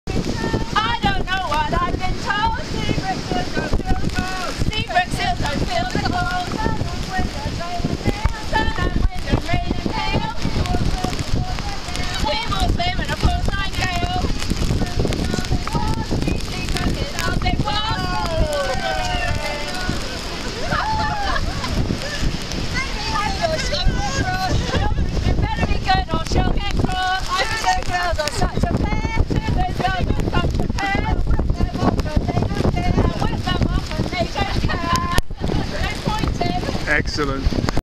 The` Singing ` Seabrook Seals Swim Team
Its sunday morning. its raining, cold the sea has 9ft waves..but the Seals love  singing in the rain..;-)) loved it..keep it going ladies you are all stars...
30/10/11
Big Ricks Blog